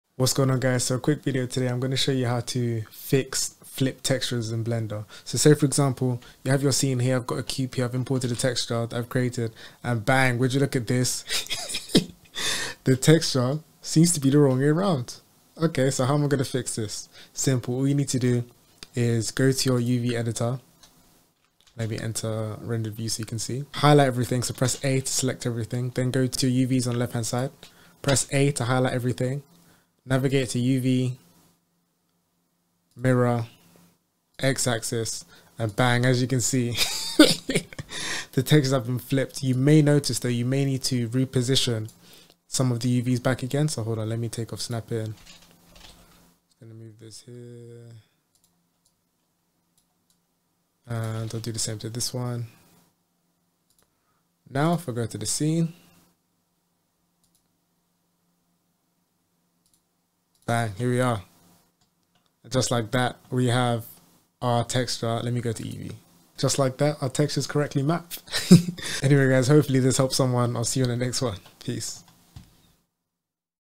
How To Fix Flipped Textures In Blender
Suuuuuper quick video today showing you a fast way to fix flipped textures & UV's within Blender!

► LUTs & Lightroom Presets
► Original Artwork, Stickers, Prints

🎙My Gear!